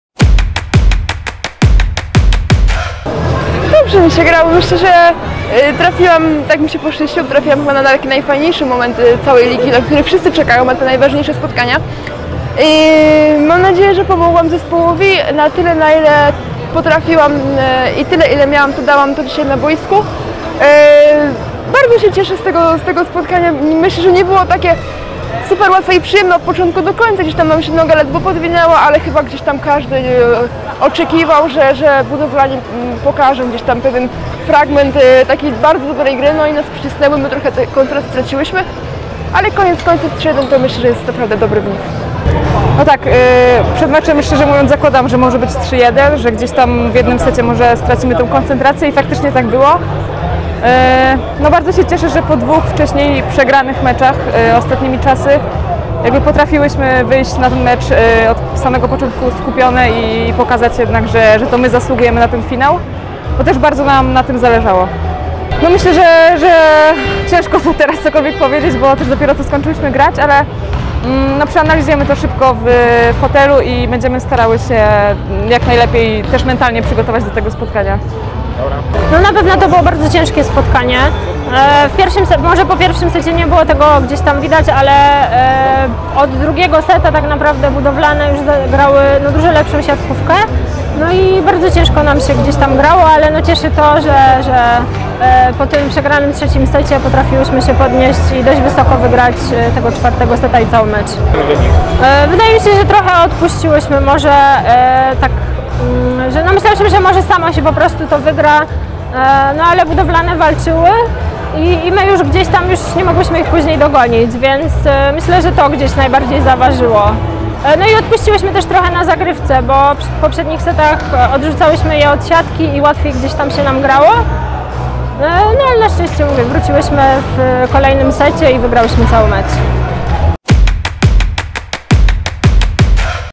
Developres BELLA DOLINA Rzeszów 3:1 Grot Budowlani Łódź | Półfinał Pucharu Polski Kobiet |26.03.2022
Półfinał Pucharu Polski Kobiet 2022 | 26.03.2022
KS DevelopRes Rzeszów 3:1 Grot Budowlani Łódź

Wywiad:
✅Malwina Smarzek
✅Aleksandra Szczygłowska
✅Katarzyna Wenerska

Zapraszamy🎥
Zapraszamy do śledzenia⤵️